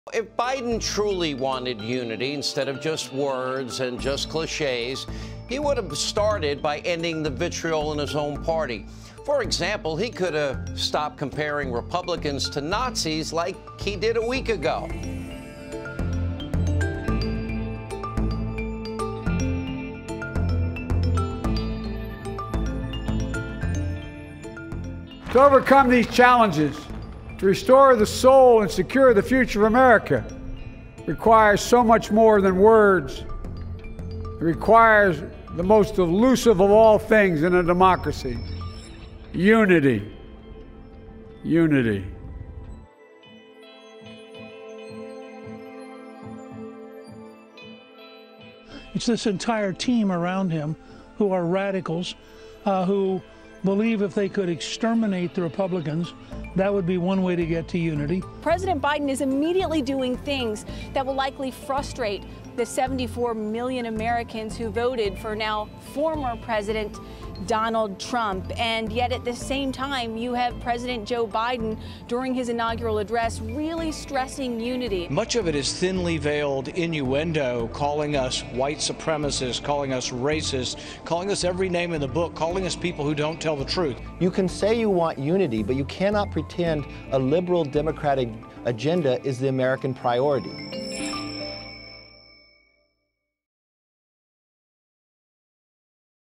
Republicans Slam Biden’s ‘Unity’ Message
President Joe Biden called for unity in his inaugural address, but some Republican figures are slamming his message.